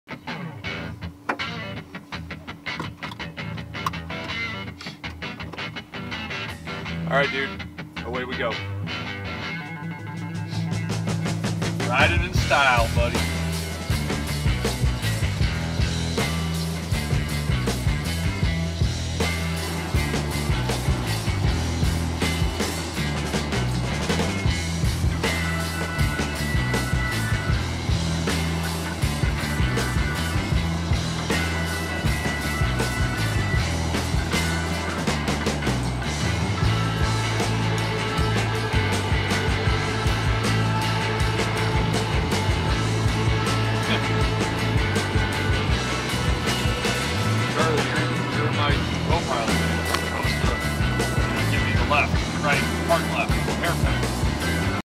Parrots In Porsches Just Him & I Driving Along
Chuck and I driving along, nothing particularly interesting, just him and I in the car. No funny business